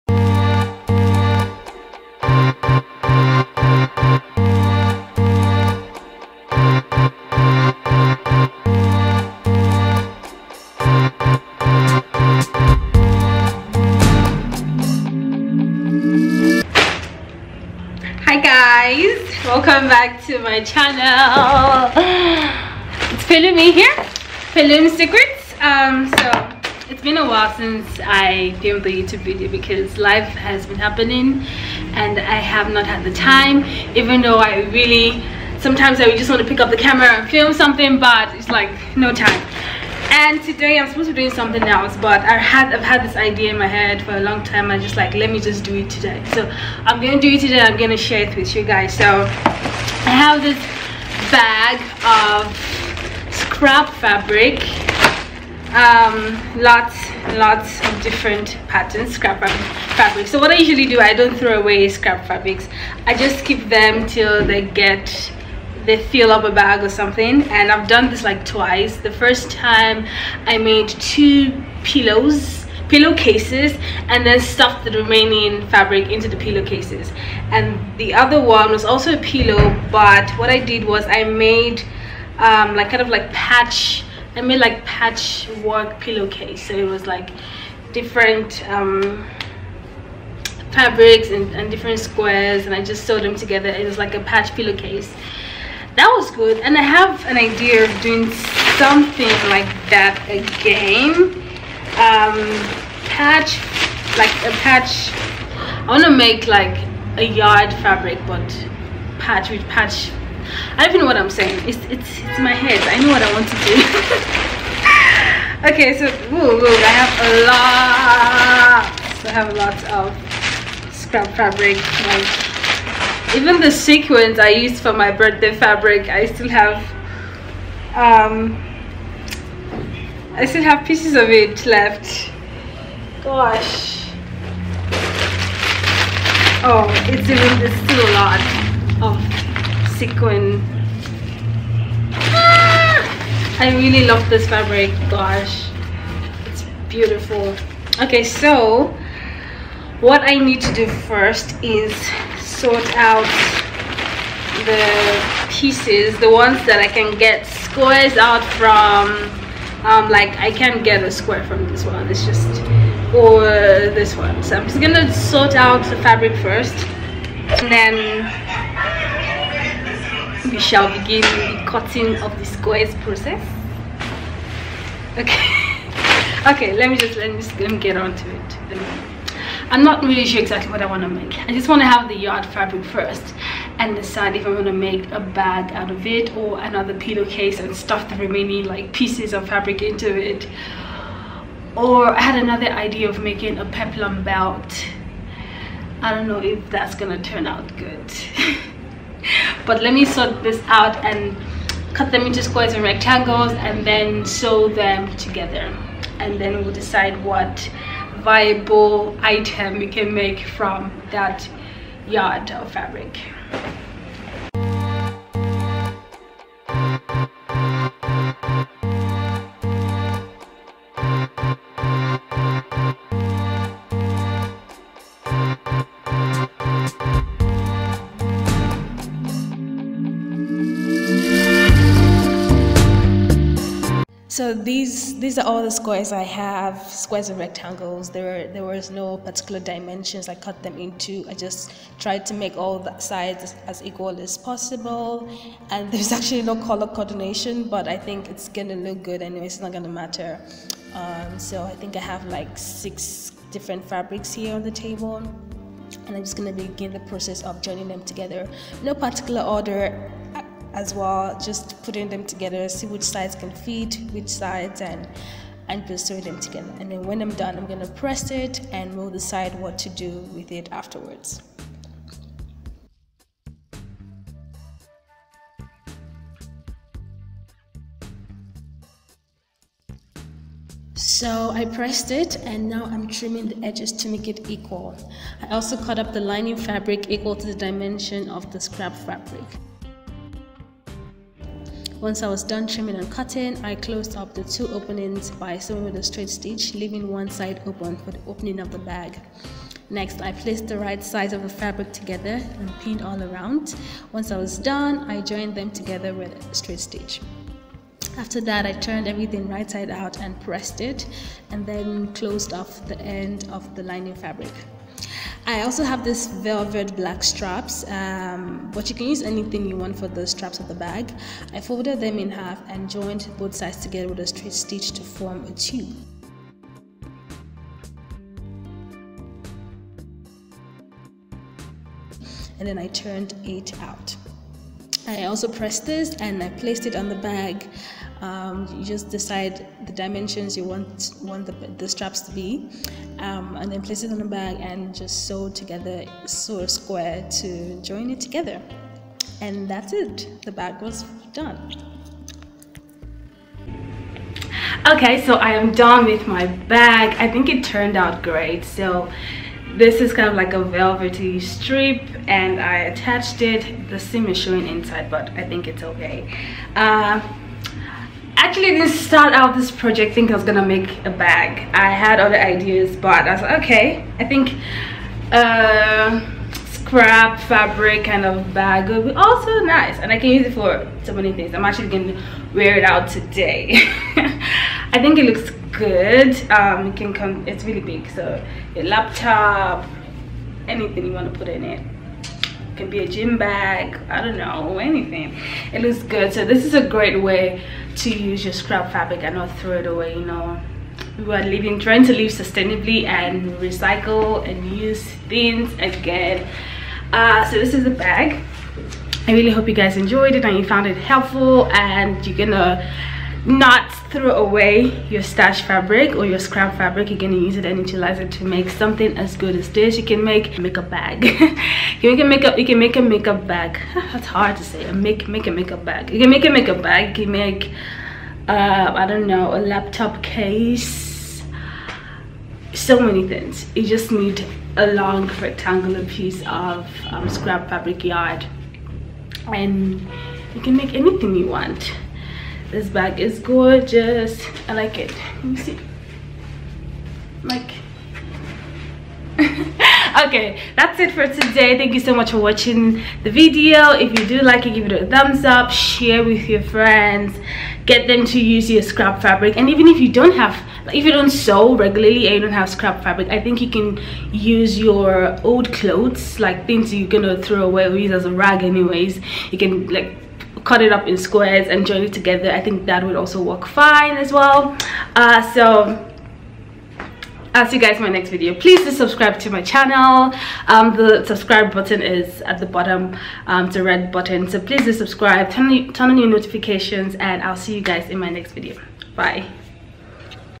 HOW TO SEW PATCHWORK TOTE BAG | Scrap fabric sewing project
What to do with Scrap Fabric? DIY Scrap Fabric Tote/Shopping Bag
Diy fabric scraps into Beautiful Handbag
In today's video, I am going to show you an awesome way to reuse scrap fabric/old clothes. It is a very easy tote bag using scrap fabric yardage.
I do not throw away my scrap fabrics, I pile them in a bag and when it's overflowing, I recycle them into something new and useable. We are all about no waste culture and recycling.

Music Credits||